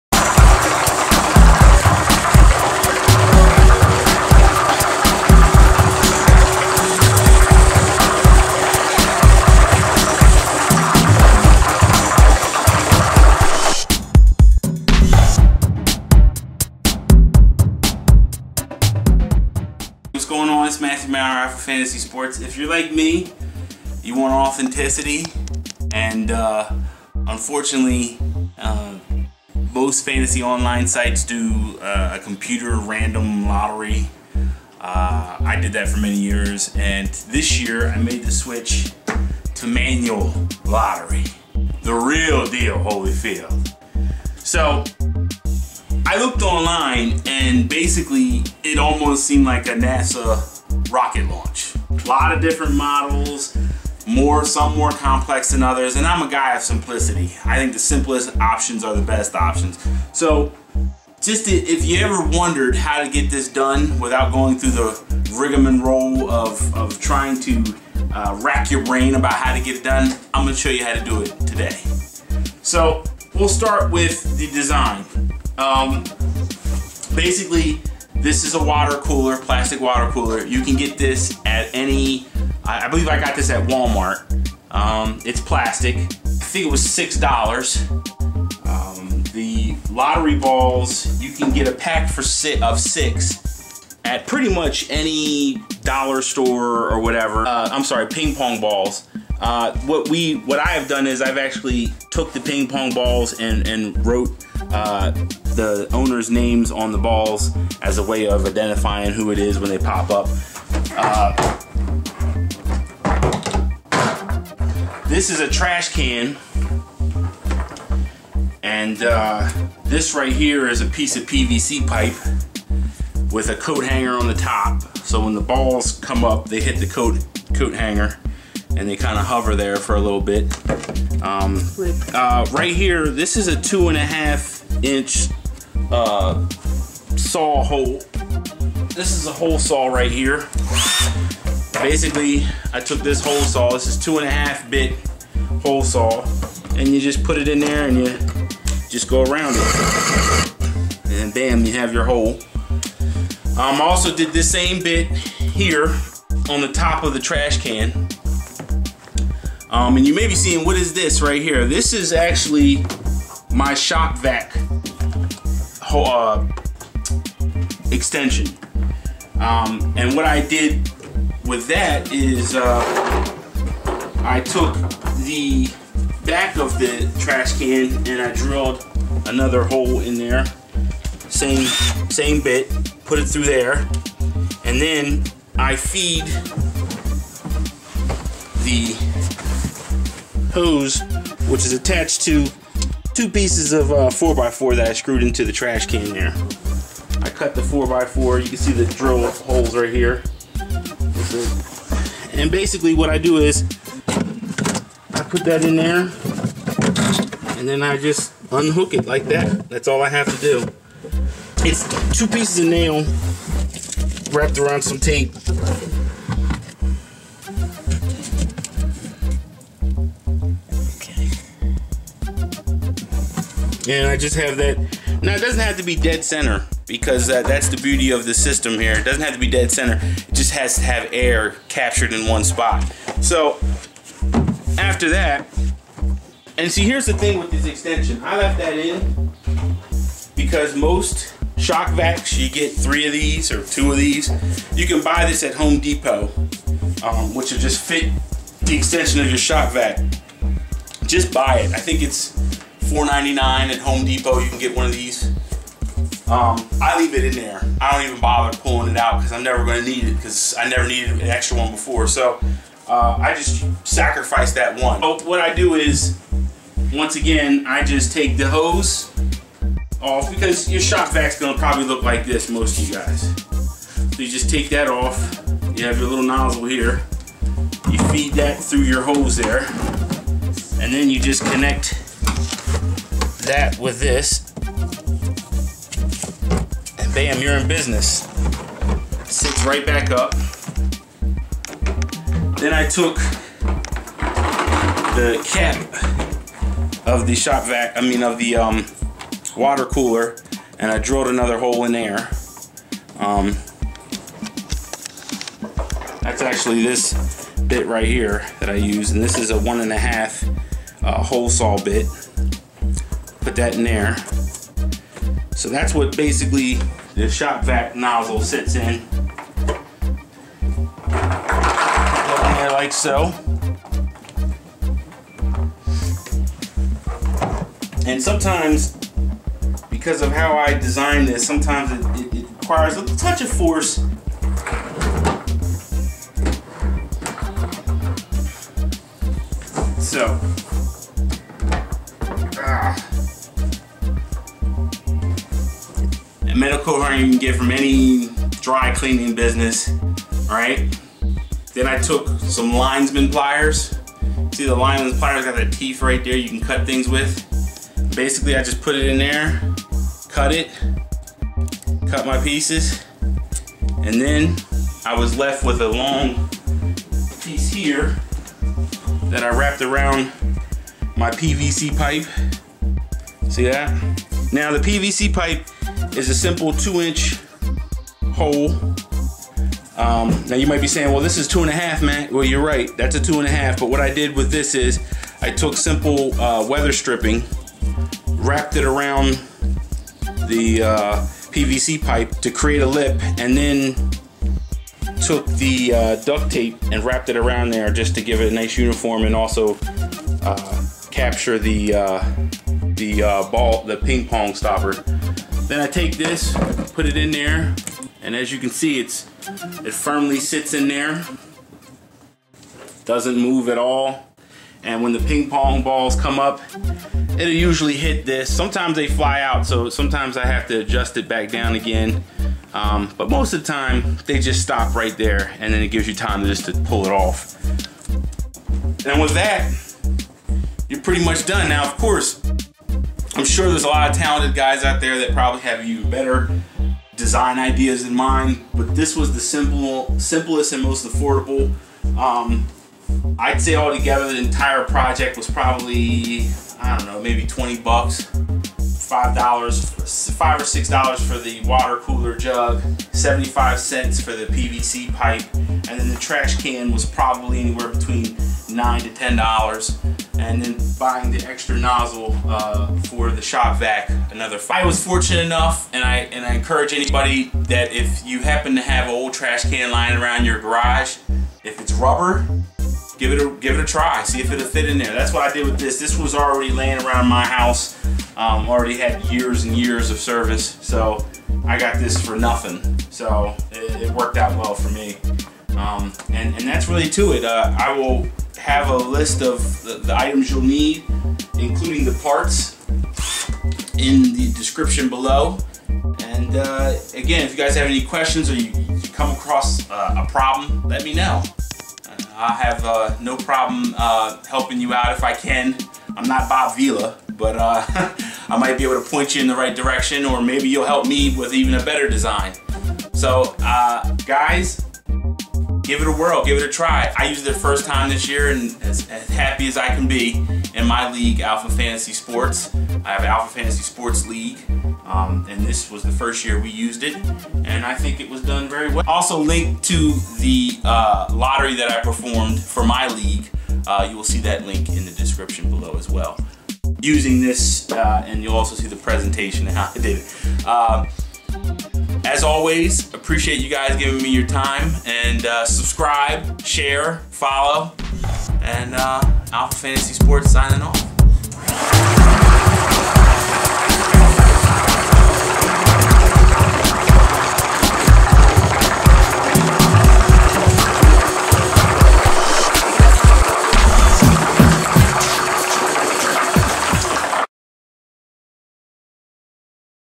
How to make Homemade Lottery Ball machine
- Alpha Fantasy football guide Matthew Maurer shows you how to make your own Lottery Ball Machine! For a extended view on fantasy ball be sure to check out our

Tool List:
1 Shop Vac with Blower Function
1 Shop Vac Extension Wand
1 Metal Coat Hanger
1 Linesman Pliers
3 Packs Ping Pong Balls
1 2 X 4 Wood
1 2ft Rubber Trash Can
1 Plastic Water Cooler
1 Roll Duct tape
1 2 inch PVC Coupling
1 Roll of Weather Stripping
1 Drill
3 Wood Screws
1 2-1/2 Hole Saw Bit
1 1-1/2 Hole Saw Bit